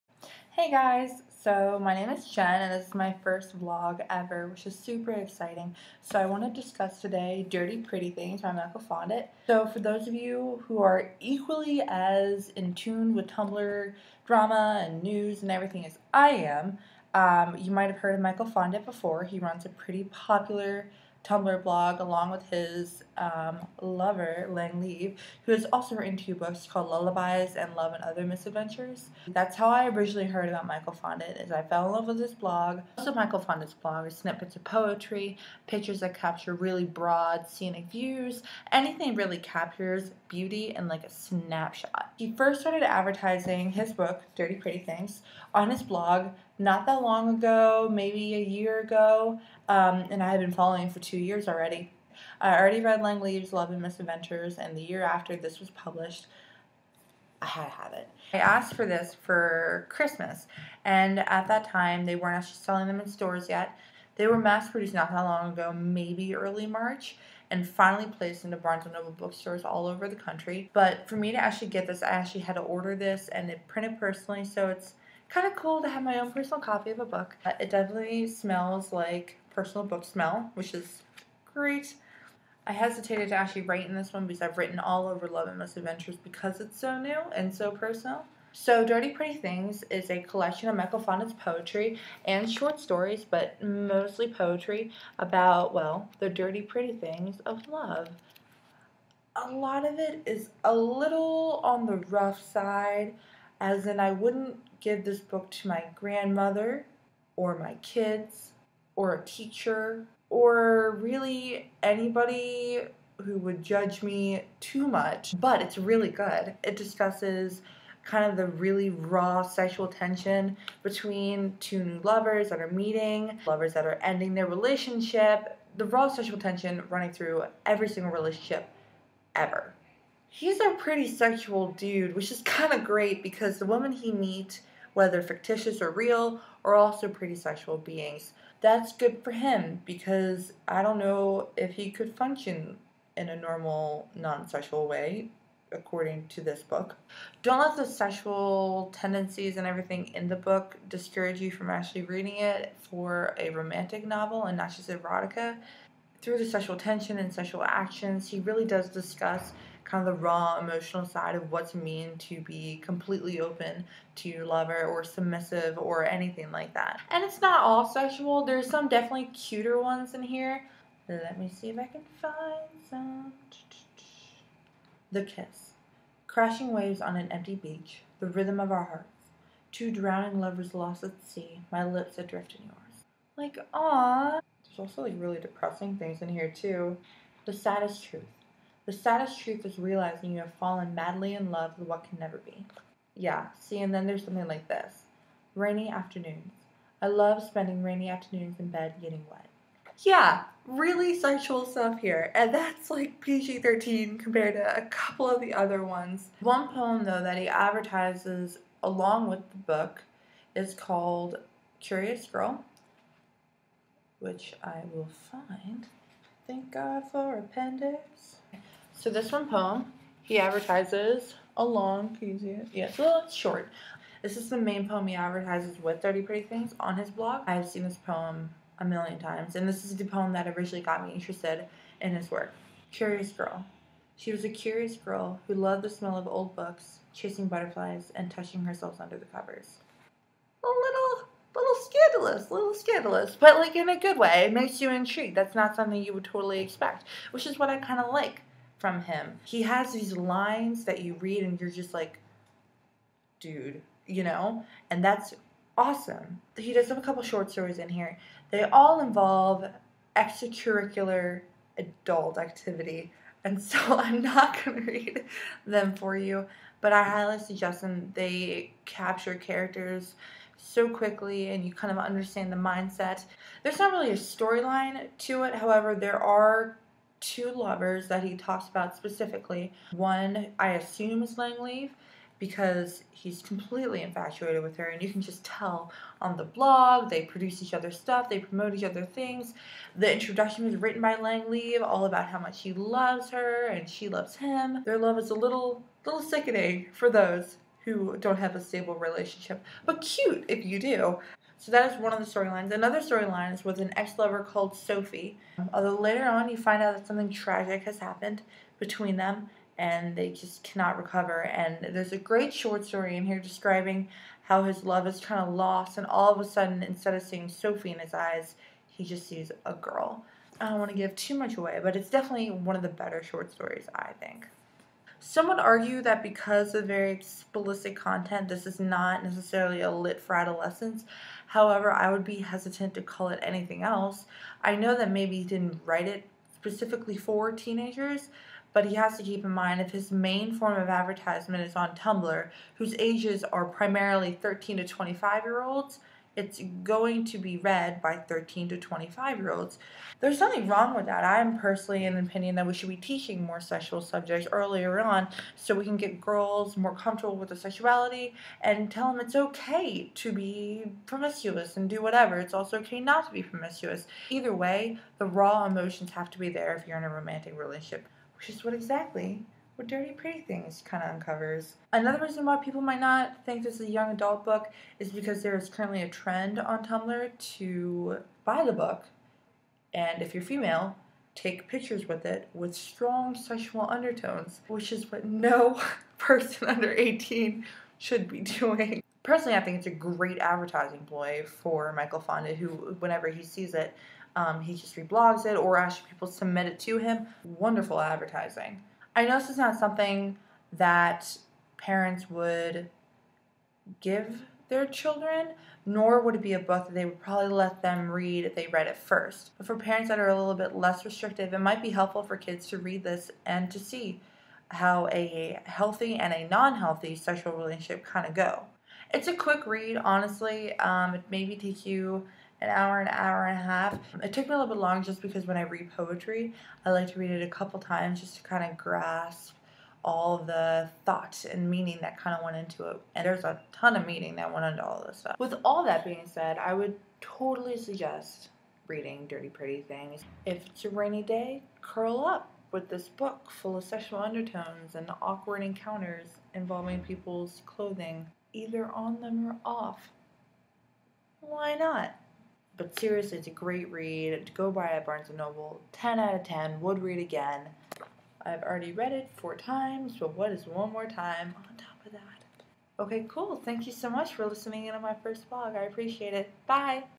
'Dirty Pretty Things' by Michael Faudet Book Review
This is my first Vlog ever! I am reviewing Dirty Pretty Things by Michael Faudet, one of my favorite poem collections at the moment. This is the first time I am doing anything like this, so bare with me.